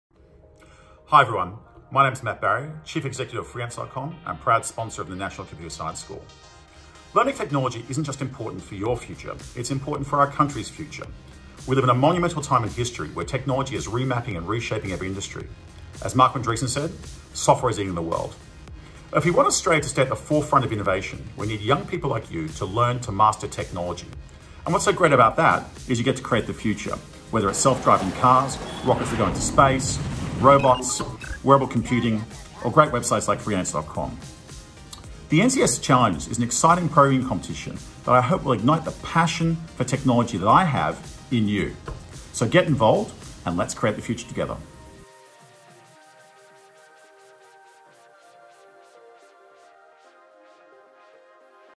Freelancer CEO Matt Barrie on how the NCSS Challenge can fuel students' passion for technology
Learn more about the NCSS Challenge, a unique programming competition for school students across all coding levels of experience, at this link: cmp.ac/challenge